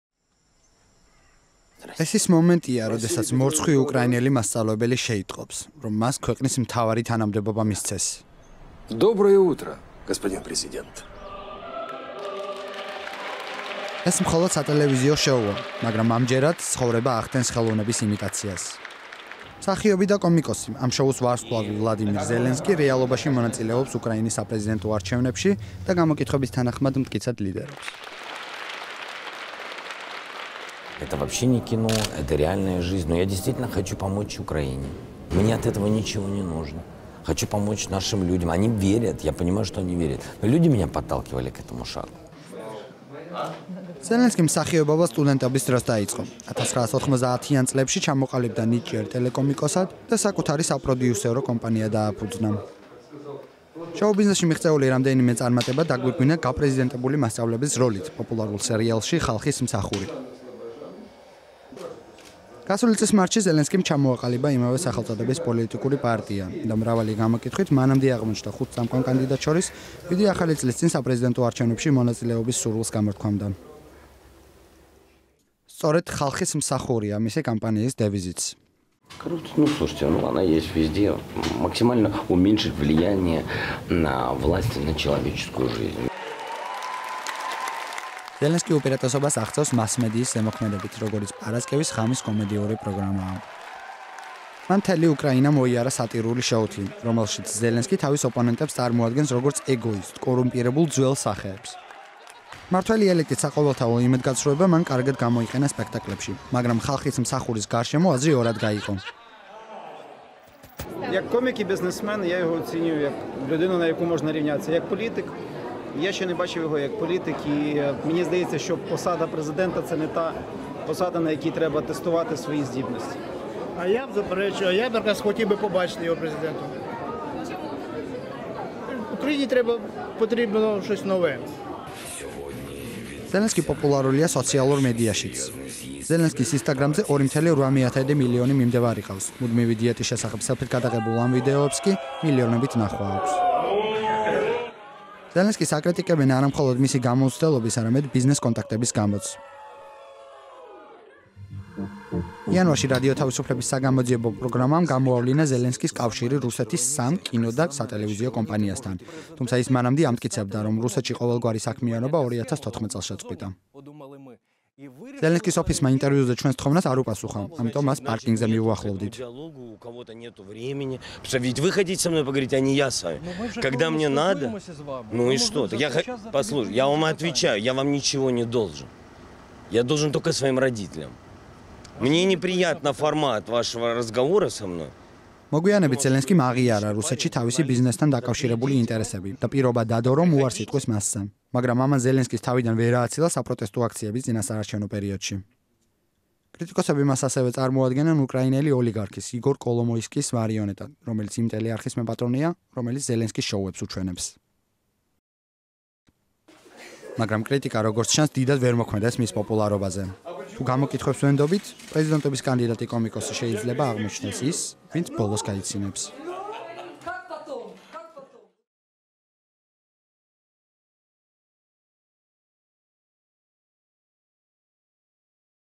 ხუმრობა იქით იყოს: უკრაინის საპრეზიდენტო არჩევნებში კომიკოსი ლიდერობს
კომიკოსი ვლადიმირ ზელენსკი უკრაინის პოპულარულ ტელეშოუში თამაშობს მასწავლებელს, რომელიც პრეზიდენტი ხდება. საზოგადოებრივი აზრის გამოკითხვა ნათელყოფს, რომ ზელენსკი რეალურ ცხოვრებაშიც ლიდერობს საპრეზიდენტო არჩევნების კანდიდატებს შორის.